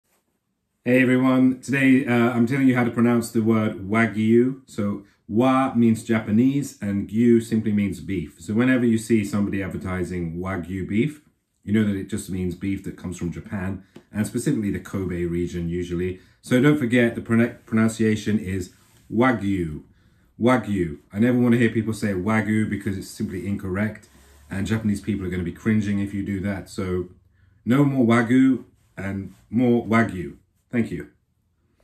You're getting it wrong!
Wagyu is of course beef from Japan. But a lot of people are not pronouncing it correctly. Here is the CORRECT way to pronounce it.